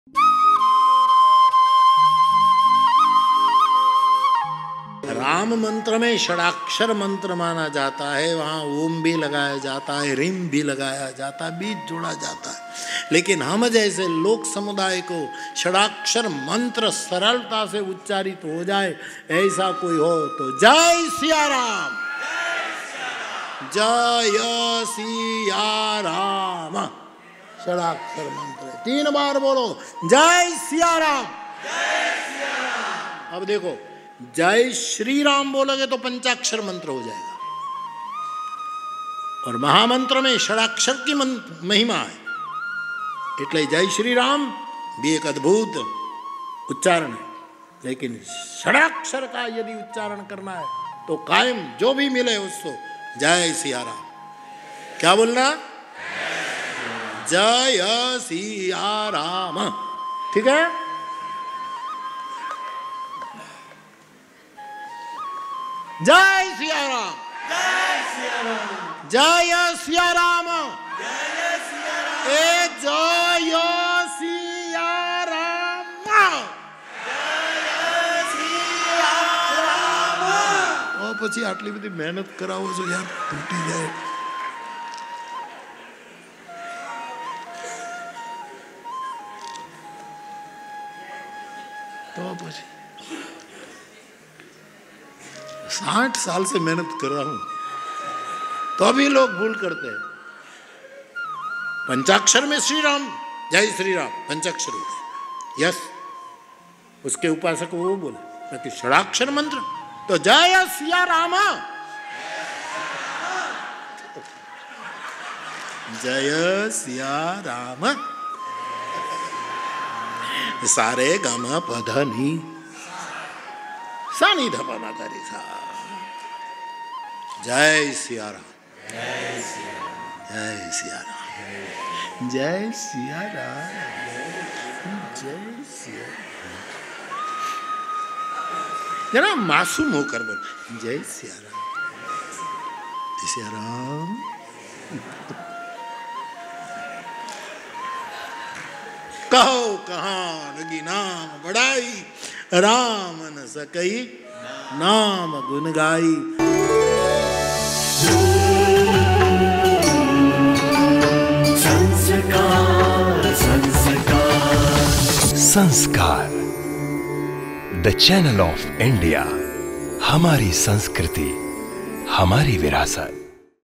जब Morari Bapu ने दिया 6 अक्षरों का मंत्र, तब जय सियाराम से गूंज उठा Ram Katha का पंडाल | MorariBapu
जब Morari Bapu ने दिया 6 अक्षरों का मंत्र, तब "जय सियाराम" से गूंज उठा Ram Katha का पंडाल